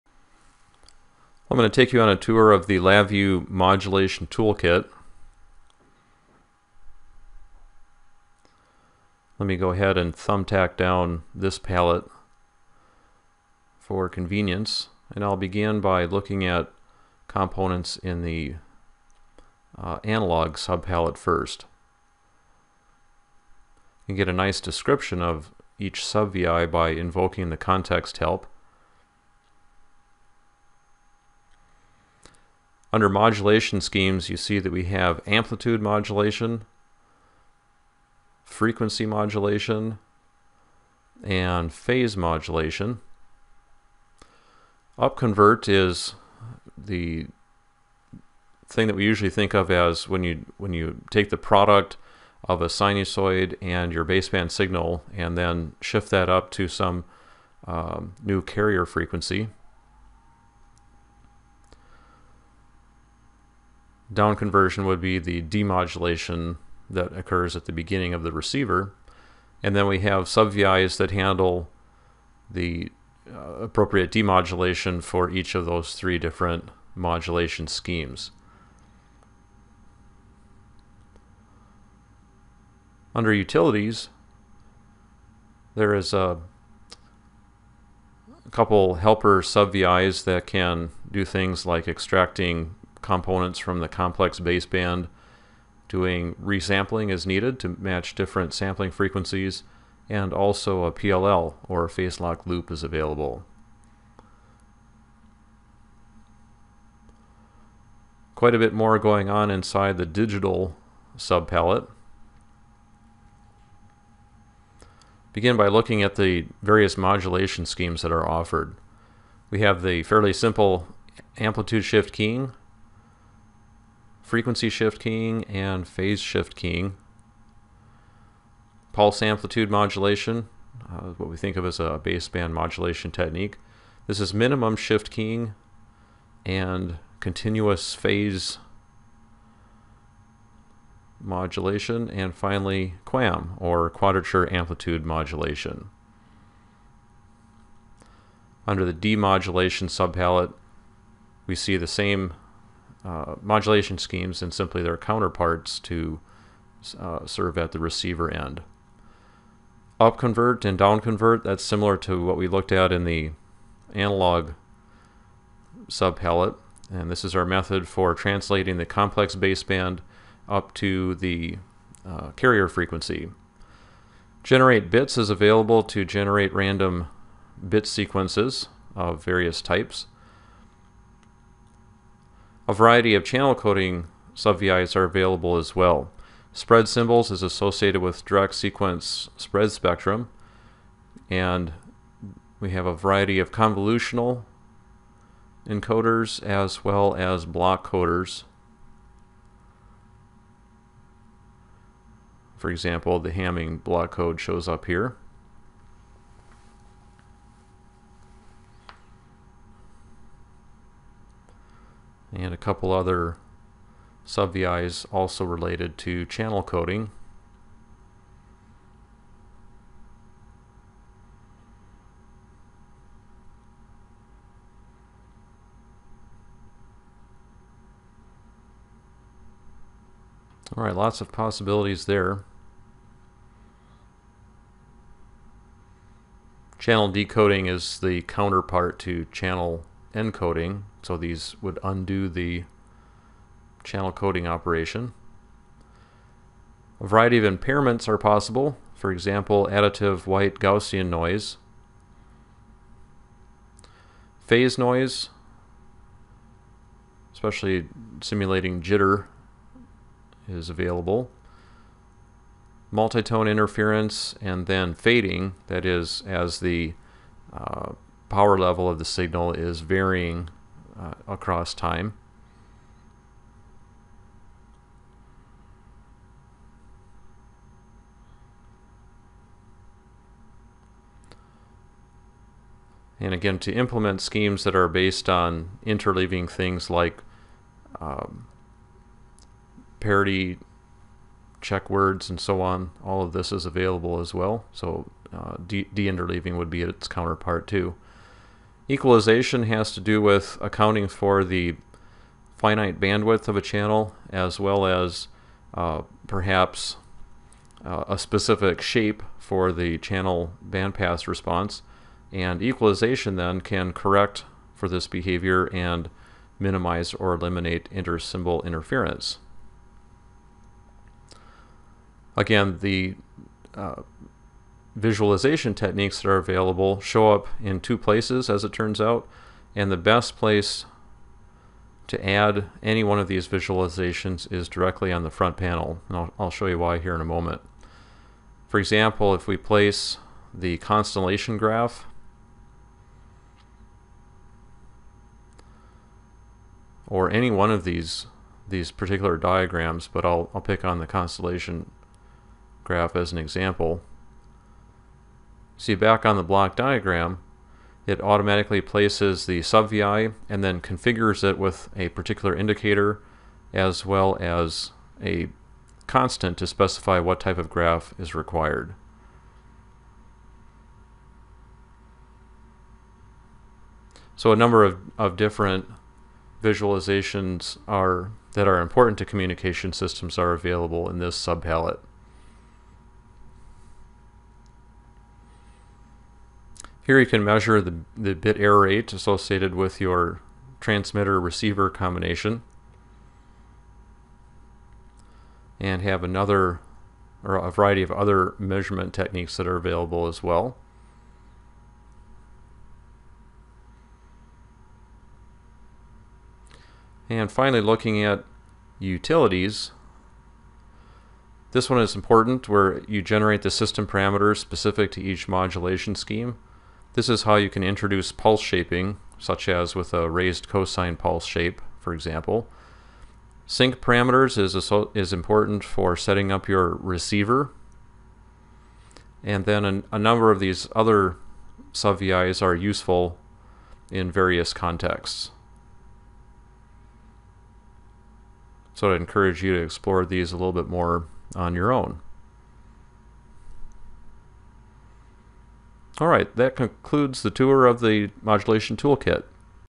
LabVIEW Modulation Toolkit: Tour of the LabVIEW Modulation Toolkit subVI palettes
Tour of the LabVIEW Modulation Toolkit subVI palettes. in the "Communication Systems Projects with LabVIEW" resource